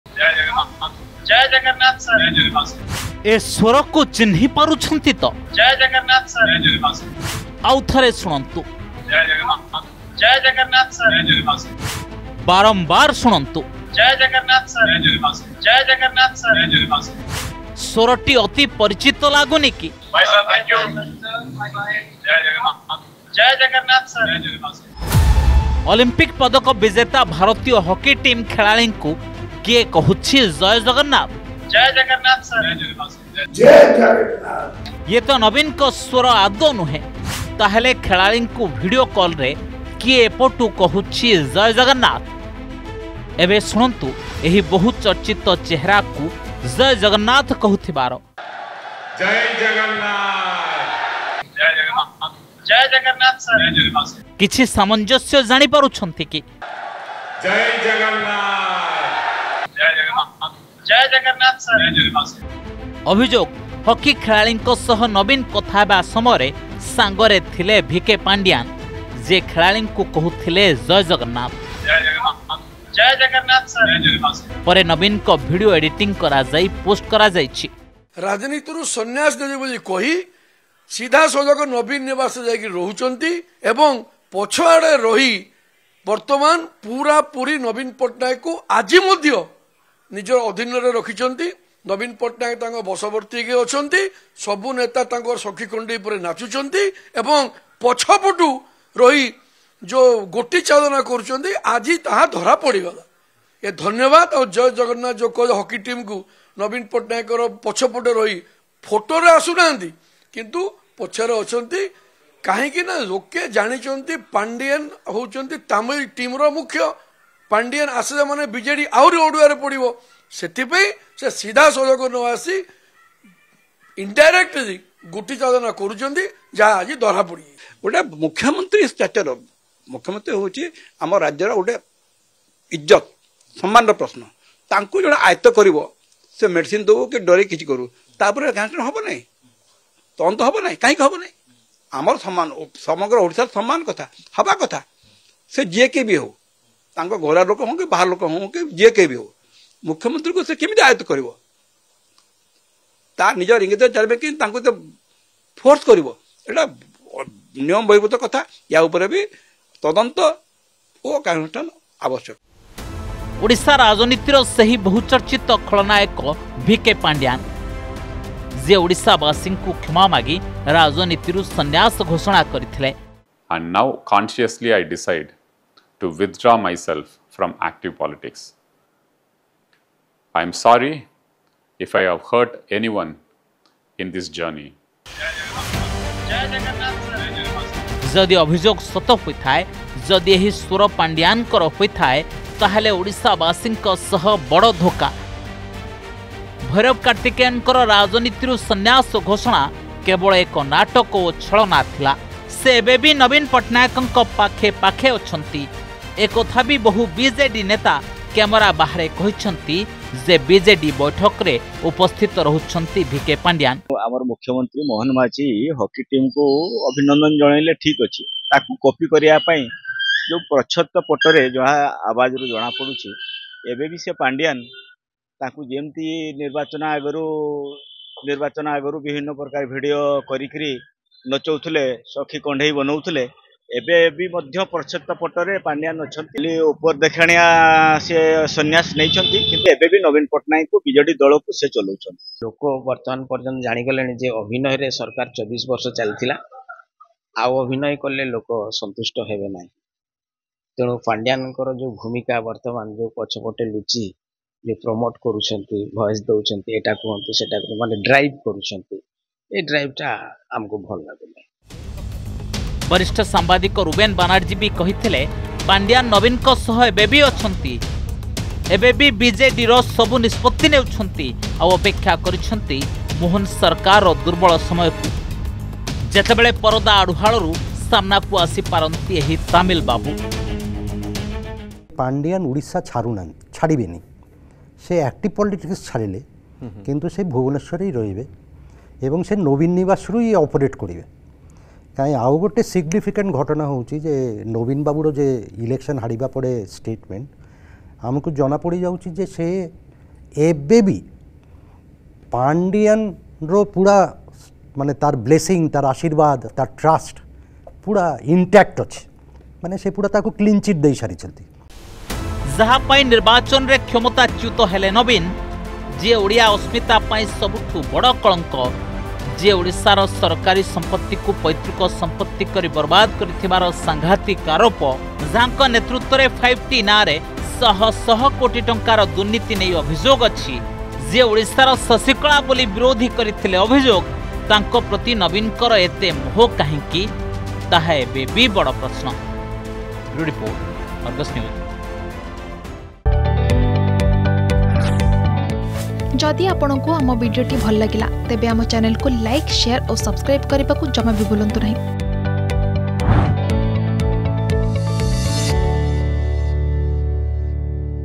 ଓଡ଼ିଶାବାସୀଙ୍କ ସହ ବିଜେଡି କରୁଛି କି ବଡ଼ ଧୋକା ? || Pandian In Hockey Player
ଓଡ଼ିଶାବାସୀଙ୍କ ସହ ବିଜେଡି କରୁଛି କି ବଡ଼ ଧୋକା । ପାଣ୍ଡିଆନଙ୍କ ରାଜନୀତିରୁ ସନ୍ୟାସ ଘୋଷଣା କରିବା ଥିଲା କି ଛଳନା ଓ ନାଟକ । ଅଲିମ୍ପିକ୍ସ ପଦକ ବିଜେତା ହକି ଟିମ୍ ସହ ନବୀନ ପଟ୍ଟନାୟକଙ୍କ କଥା ହେବା ଭିଡ଼ିଓରେ ଥିବା ଜୟ ଜଗନ୍ନାଥ ସ୍ବରକୁ ନେଇ ବଡ଼ ଅଭିଯୋଗ ।  ପୂର୍ବ ଭଳି ନବୀନଙ୍କ ସହ ଛାଇ ଭଳି ରହୁଛନ୍ତି ପାଣ୍ଡିଆନ । ଏବେ ବି ପଛ ପଟେ ରହି ବିଜେଡିକୁ ପରିଚାଳନା କରୁଛନ୍ତି ଭୈରବ କାର୍ତ୍ତିକେୟନ ।